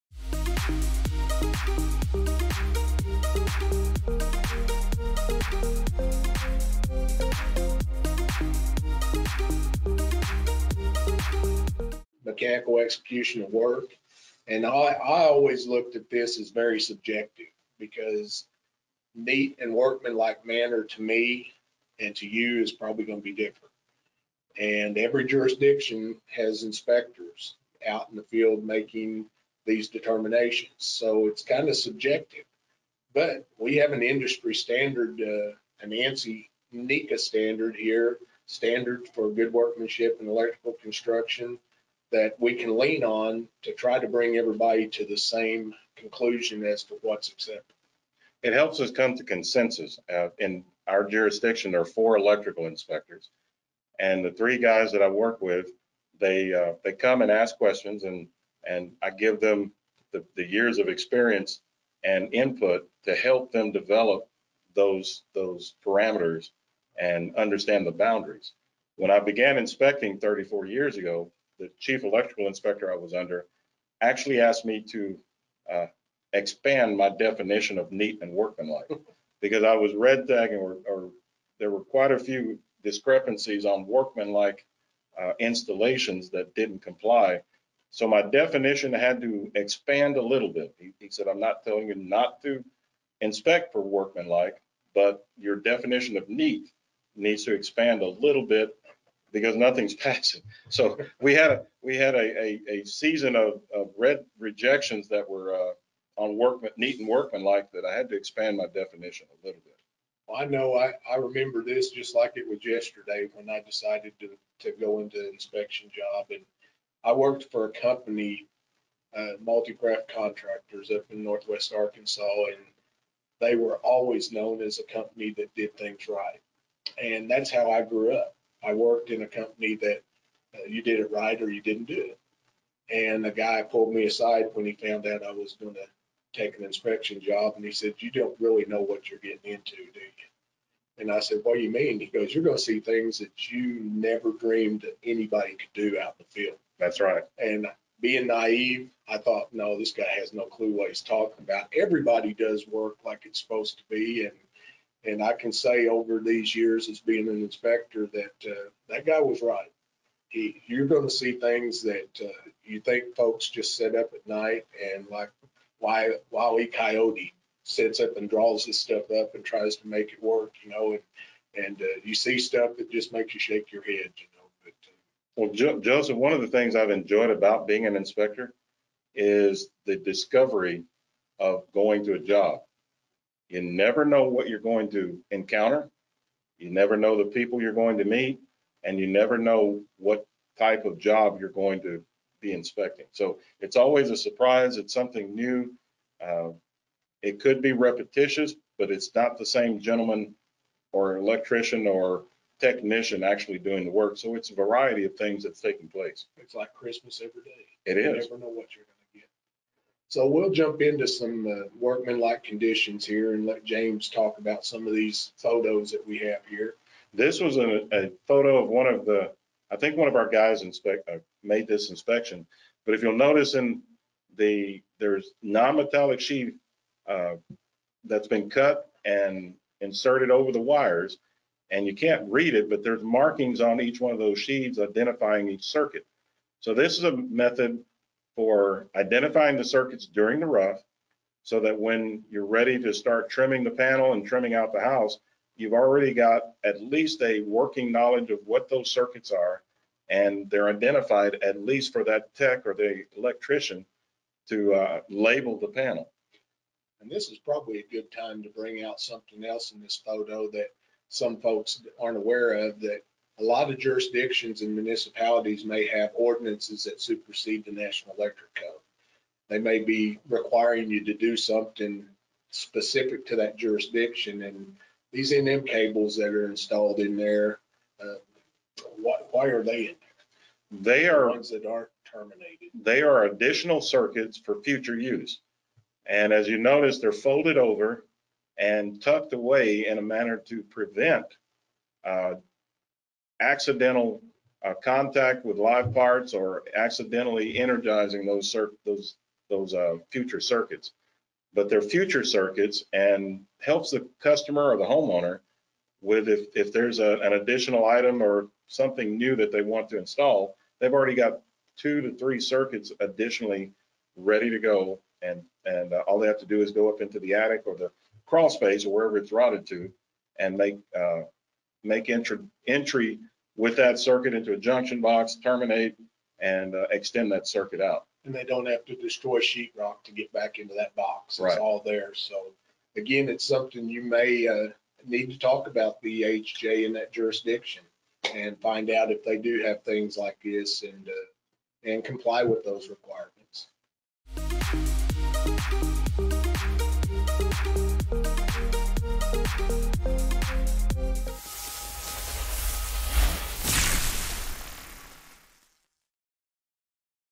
IAEI Training Shorts — Mechanical Execution of Work
What exactly is a workmanlike manner? City of Irving Chief Electrical Inspector James Hathorn and IAEI's Director of Technical Education discuss proper mechanical execution of work that's important on the job site to the local inspector. To view the full training program, register